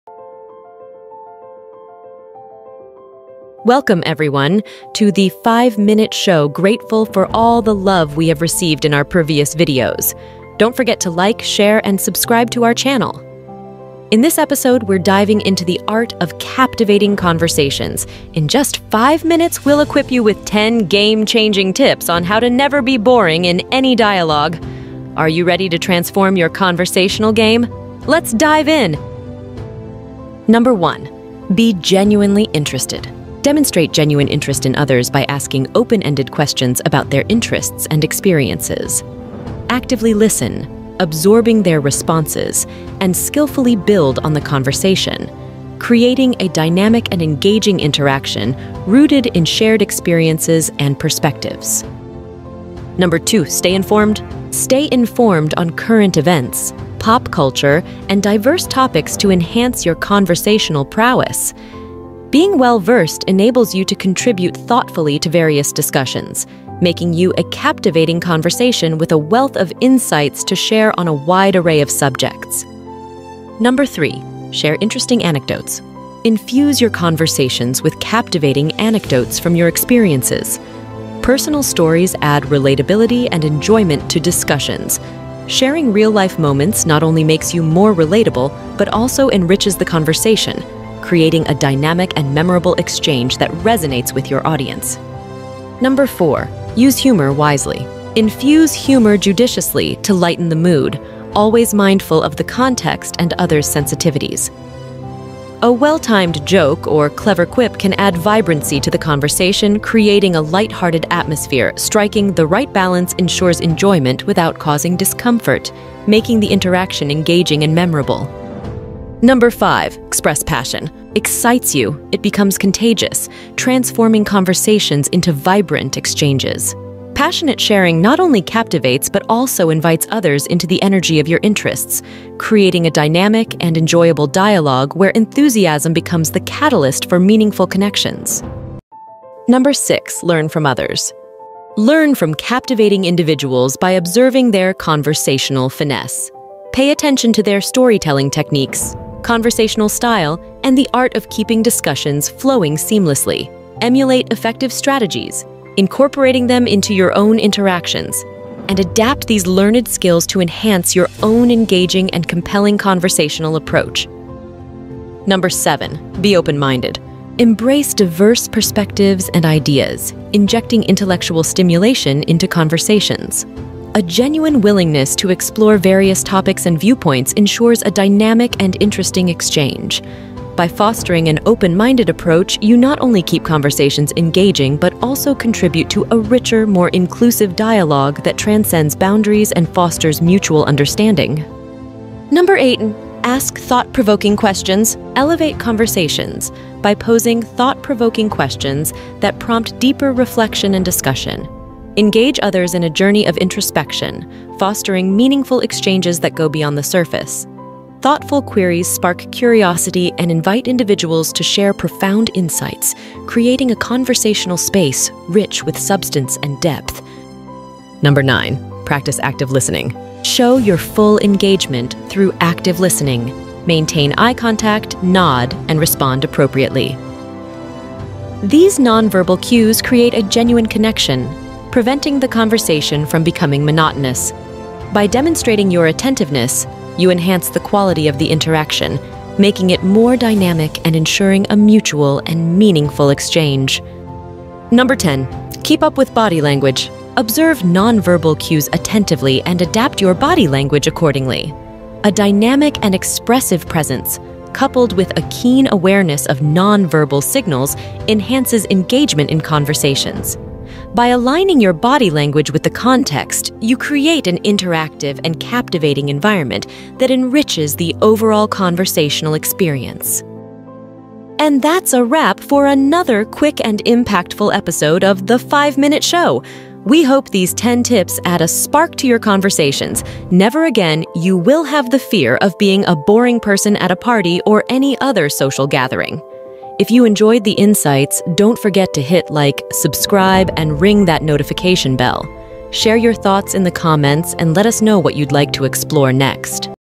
Are You A Boring Person? NEVER BE BORING AGAIN | Under 5 Minutes
Well, Some of us deal the situation where we think we are the most boring person in the Party and nobody would talk to us. But not anymore!